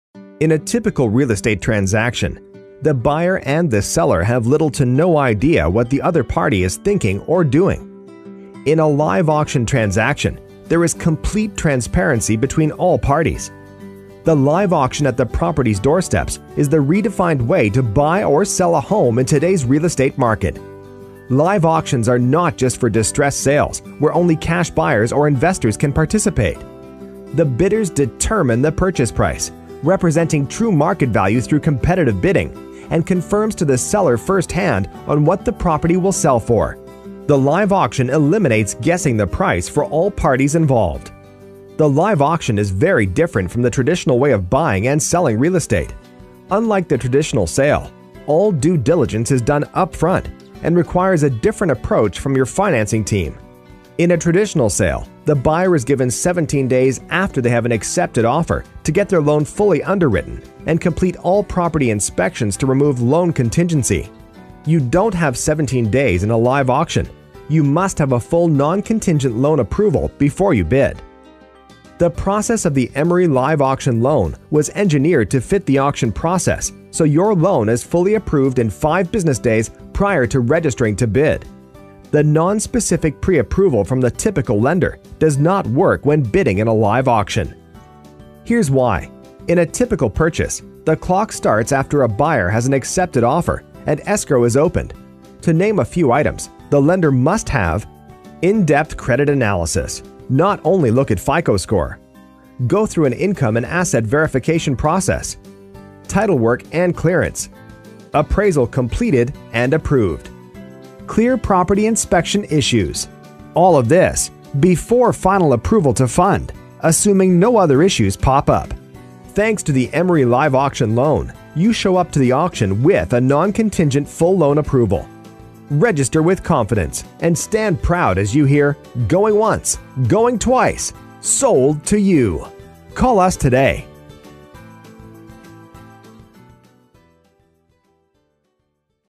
Auction Lending for Auctions
Auctions are not limited to only cash buyers! Check out this video to learn more about financing your purchase of an Auction property.